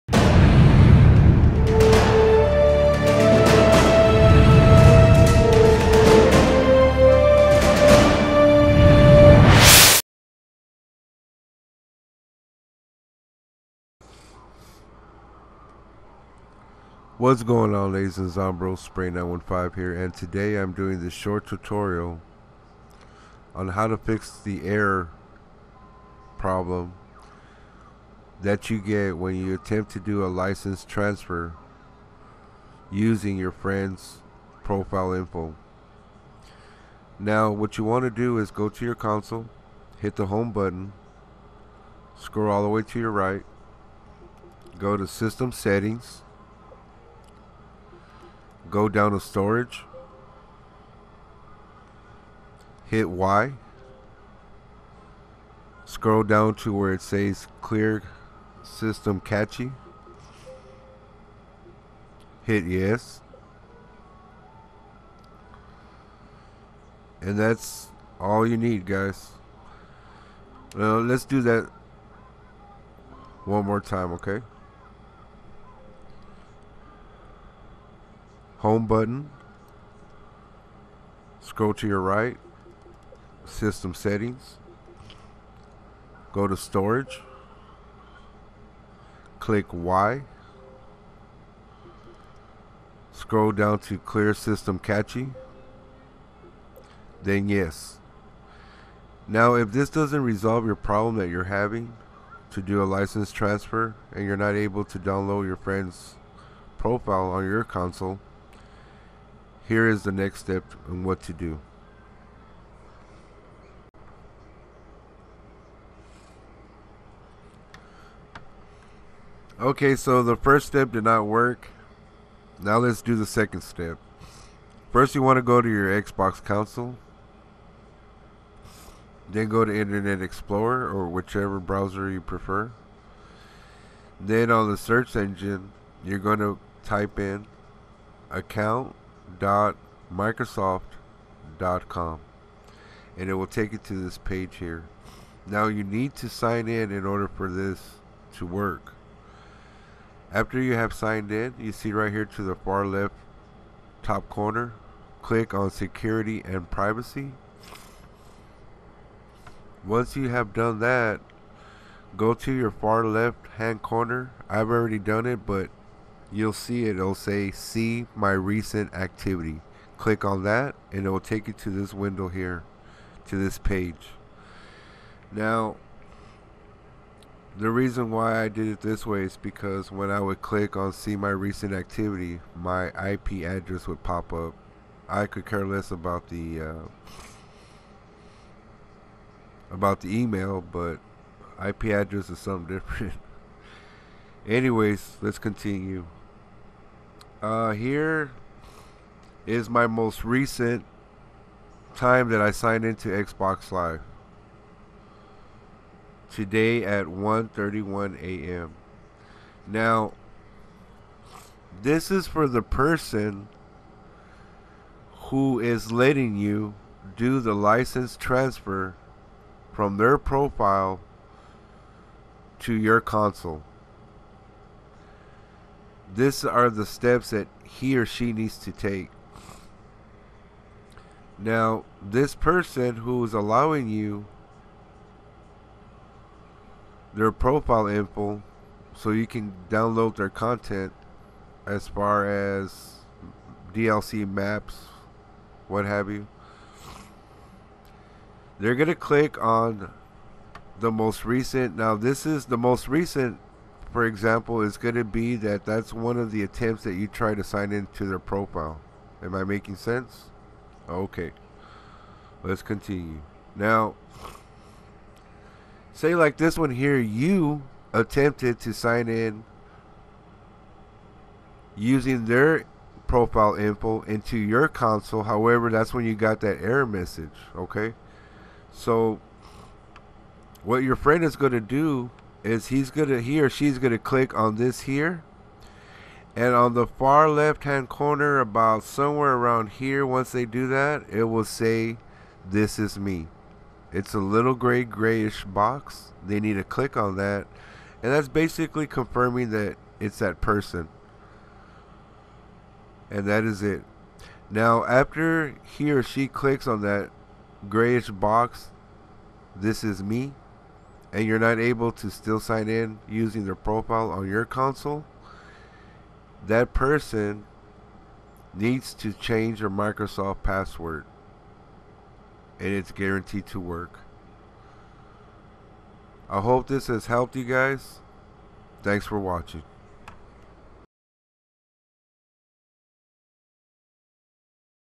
ERROR CODE 80151012 FIX ON XBOX 360
CORRECTION ON VIDEO!!! YOU WILL SEE A BLUE BOX THAT SAY'S "THIS WAS ME"